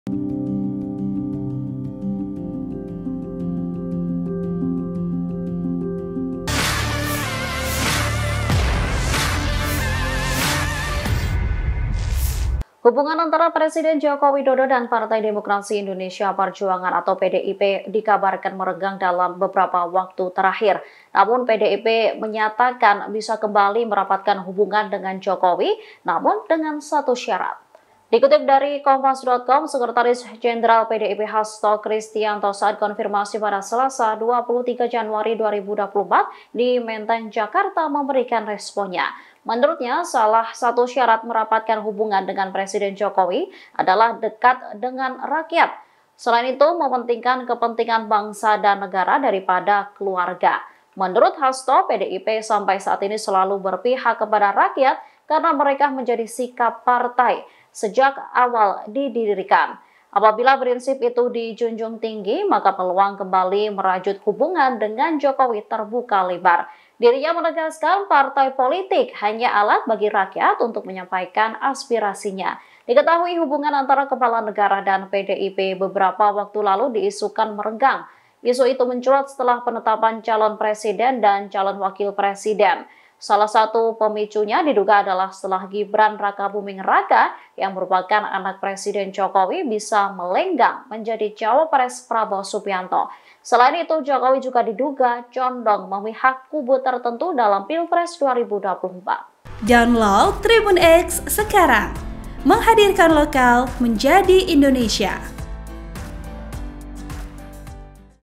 PDIP Melunak Siap 'Terima' Jokowi dengan 1 Syarat: Utamakan Kepentingan Rakyat di Atas Keluarga
TRIBUN-TIMUR.COM - TRIBUN-VIDEO.COM - Hubungan antara Presiden Joko Widodo dan Partai Demokrasi Indonesia Perjuangan (PDI P) dikabarkan merenggang dalam beberapa waktu terakhir.

Namun, PDIP menyatakan membuka kembali merapatkan hubungan dengan Jokowi, namun dengan satu syarat.

Sumber: Tribunnews

(TRIBUN-TIMUR.COM)